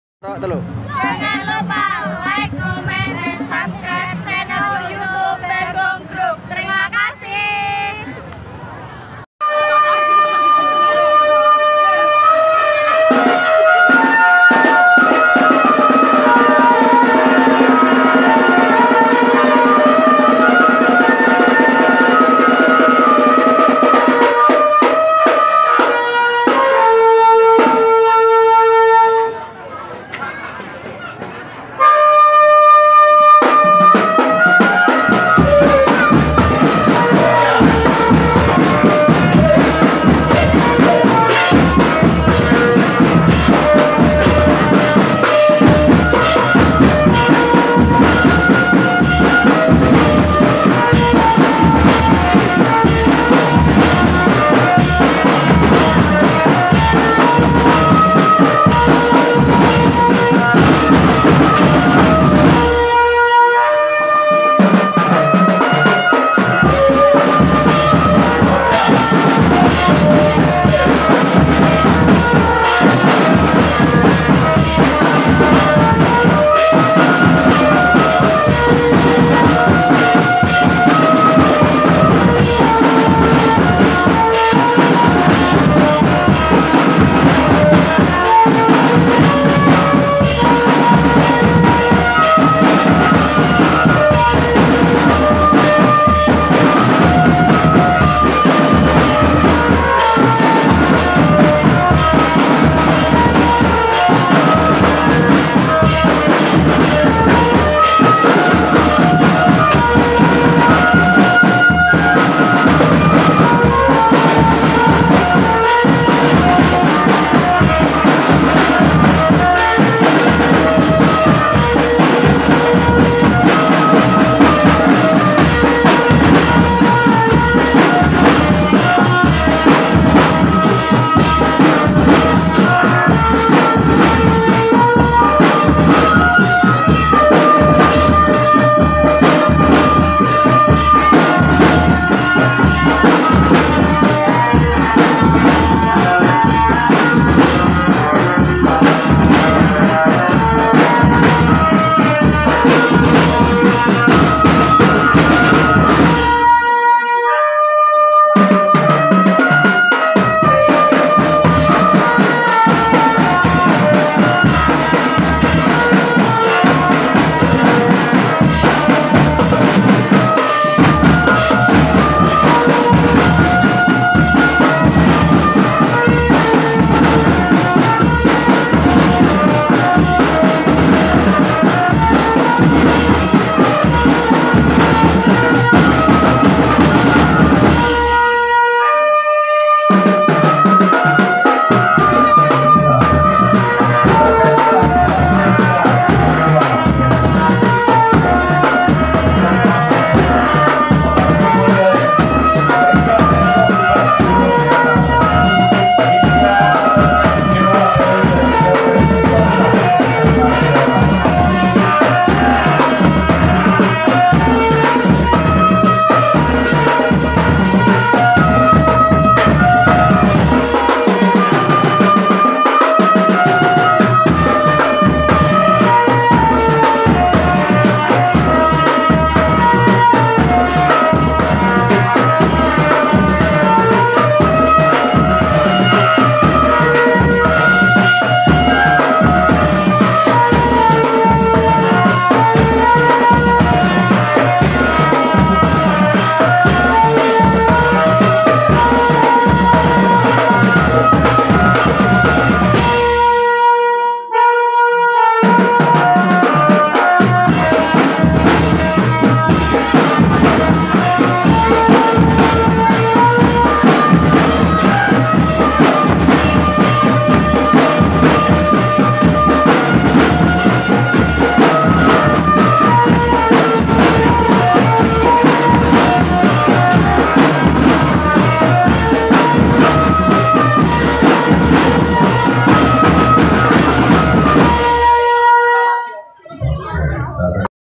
IKI PAS LIVE KEPOH PIDODO POKO'E MANTUL TENAN || BAGONG Group
demak 27 OKTOBER 2021 DEWA DEWI LIVE DESA KEPOH PIDODO

TRIMAKASIH ATAS KUNJUNGANYA

TUNGGU PERFOM DEWA DEWI SELANJUTNYA DI CHANNEL YOUTUBE BAGONG Group